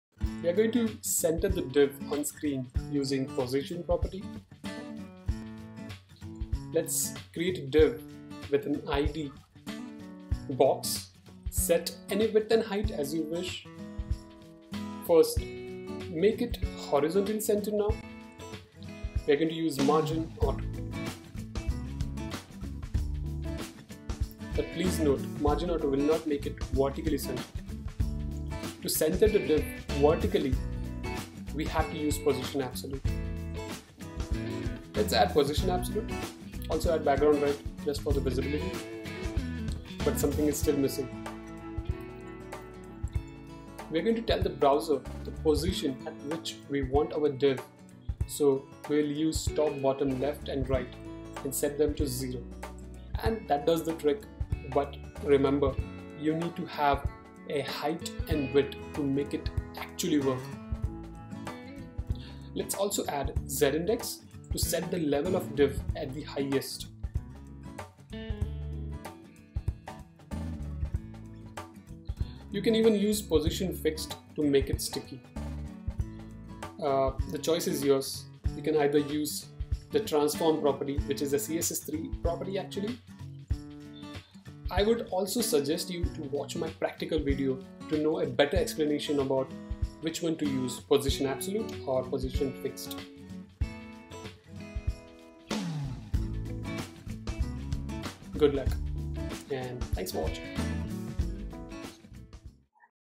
03 Center the BOX / DIV on screen :: CSS
We are going to center the DIV on screen using CSS Position property.